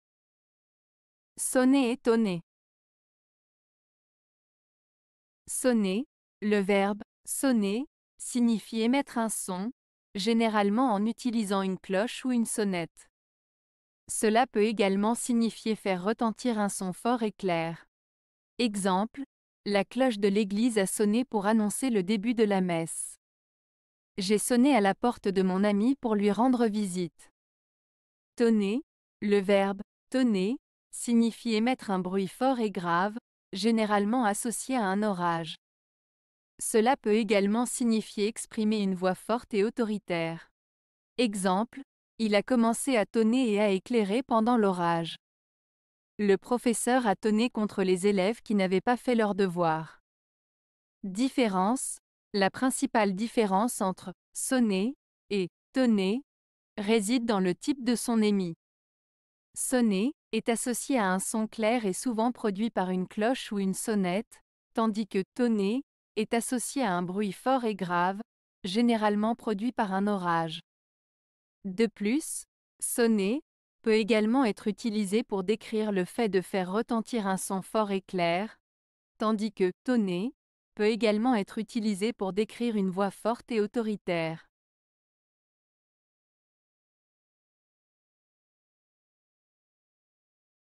Sonner et Tonner (Une Lettre)
Sonner:
Le verbe "sonner" signifie émettre un son, généralement en utilisant une cloche ou une sonnette. Cela peut également signifier faire retentir un son fort et clair.

Exemples:
- La cloche de l'église a sonné pour annoncer le début de la messe.
- J'ai sonné à la porte de mon ami pour lui rendre visite.

Tonner:
Le verbe "tonner" signifie émettre un bruit fort et grave, généralement associé à un orage. Cela peut également signifier exprimer une voix forte et autoritaire.

Exemples:
- Il a commencé à tonner et à éclairer pendant l'orage.
- Le professeur a tonné contre les élèves qui n'avaient pas fait leurs devoirs.

Différence:

La principale différence entre "sonner" et "tonner" réside dans le type de son émis. "Sonner" est associé à un son clair et souvent produit par une cloche ou une sonnette, tandis que "tonner" est associé à un bruit fort et grave, généralement produit par un orage. De plus, "sonner" peut également être utilisé pour décrire le fait de faire retentir un son fort et clair, tandis que "tonner" peut également être utilisé pour décrire une voix forte et autoritaire.